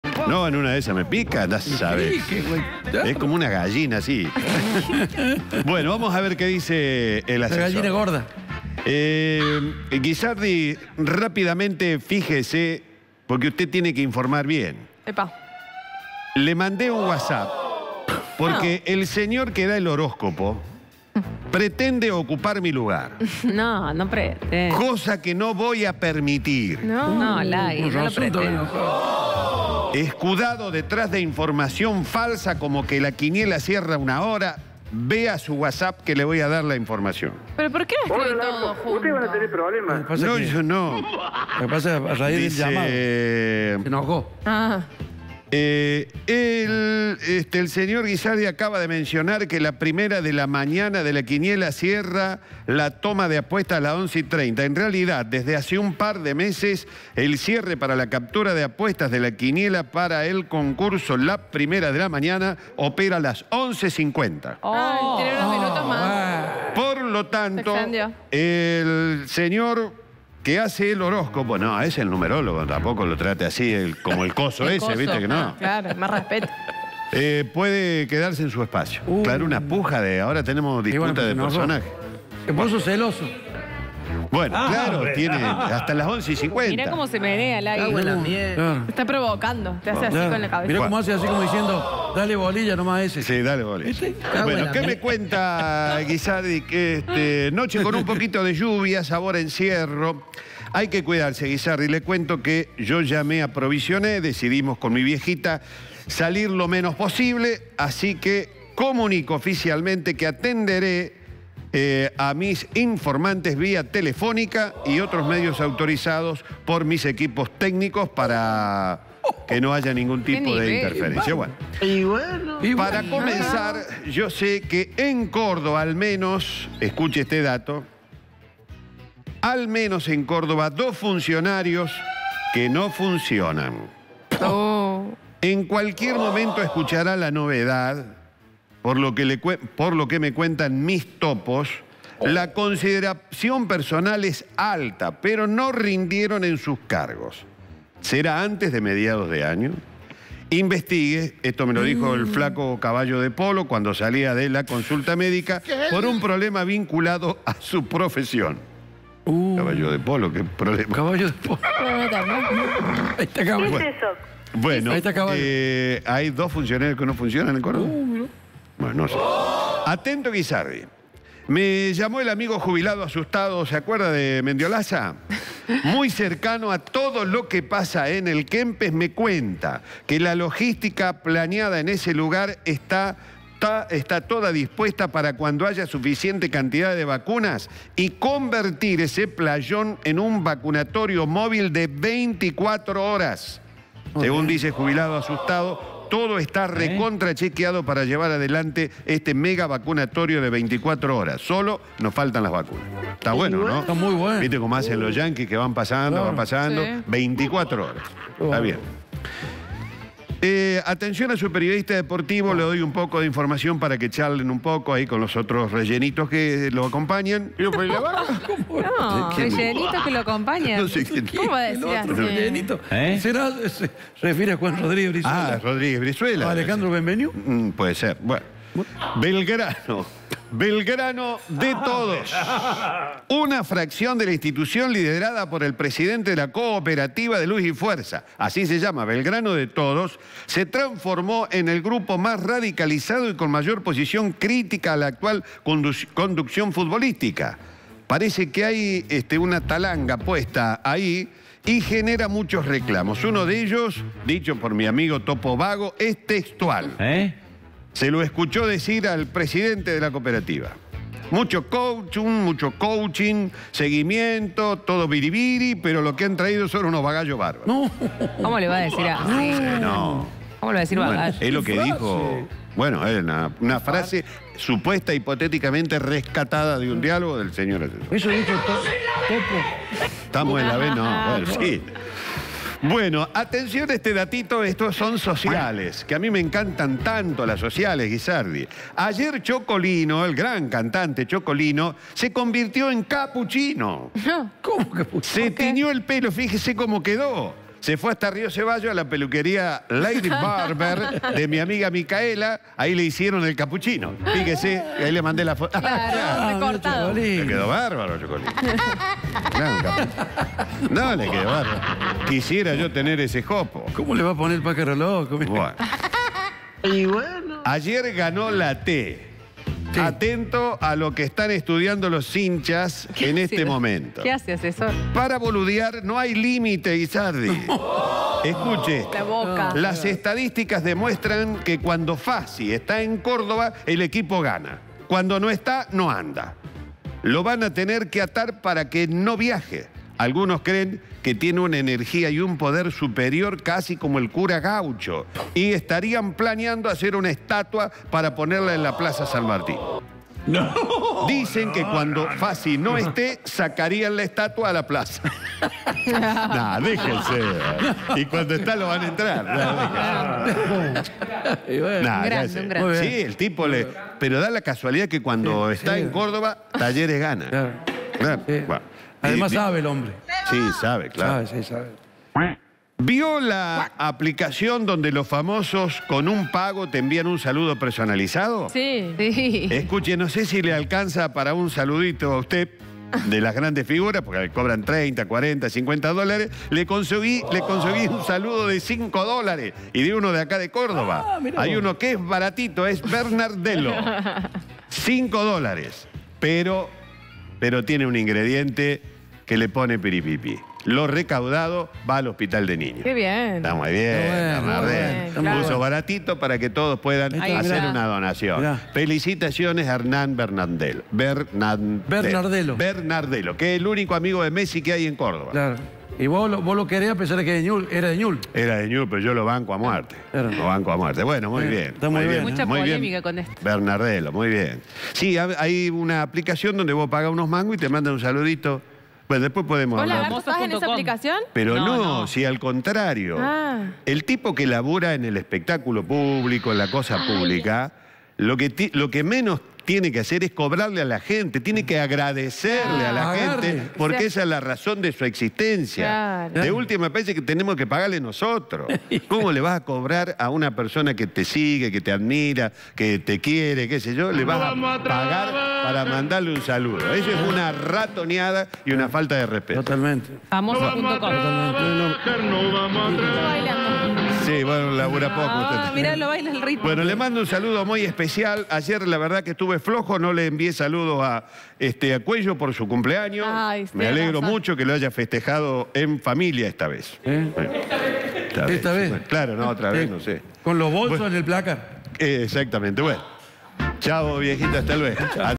Buscan sacar a personal jerárquico que no funciona en Córdoba
De acuerdo a "Caballo de Polo", hay dos funcionarios de alta jerarquía que serían desplazados de sus puestos, dado que desde el Panal evaluaron que su rendimiento no era el deseado.
Entre otras noticias, "Jubilado Asustado" mencionó que se está armando un vacunatorio 24hs. A su vez, una agrupación de Belgrano -según "Topo Vago"- estaría bastante radicalizada.
Además, las estadísticas muestran que cuando Andrés Fassi está en Córdoba, Talleres consigue ganar sus partidos, algo que ocurre de la misma forma que ocurren los milagros del Cura Brochero; el club está analizando hacer una estatua del dirigente para que le de suerte a sus jugadores de forma permanente.